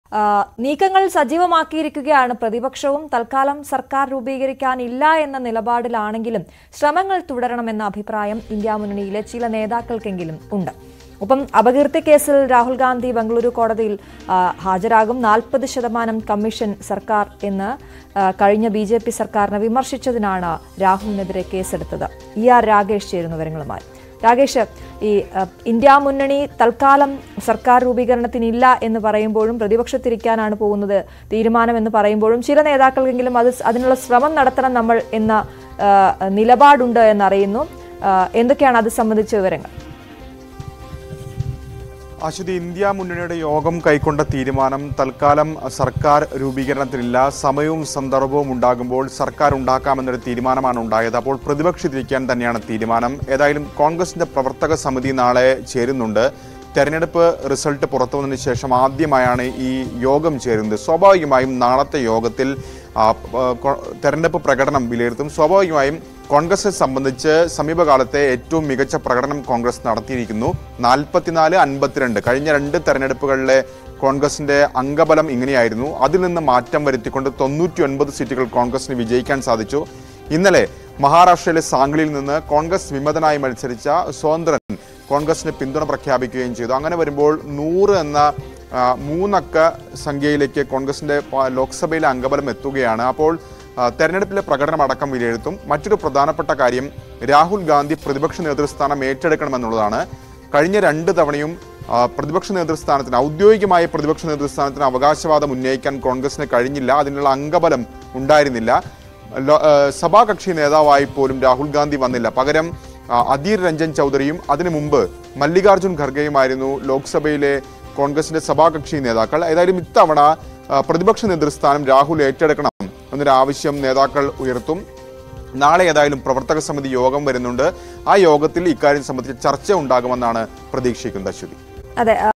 ഇന്ത്യ സഖ്യത്തിന്റെ ഇനിയുള്ള പ്ലാന്‍ എന്ത് ? | INDI Alliance | Rahul Gandhi | Mallikarjun Kharge
നീക്കങ്ങൾ സജീവമാക്കി പ്രതിപക്ഷം. തത്ക്കാലം സർക്കാർ രൂപീകരക്കാനില്ല എന്ന നിലപാടിലാണെങ്കിലും ശ്രമങ്ങൾ തുടരണമെന്ന അഭിപ്രായം ഇന്ത്യ മുന്നണിയിലെ ചില നേതാക്കൾക്കെങ്കിലും ഉണ്ട്.

The opposition has activated its moves to seize power. At least some of the leaders of the INDI Alliance are of the opinion that efforts should be continued even though they are of the view that no government can be formed for the time being.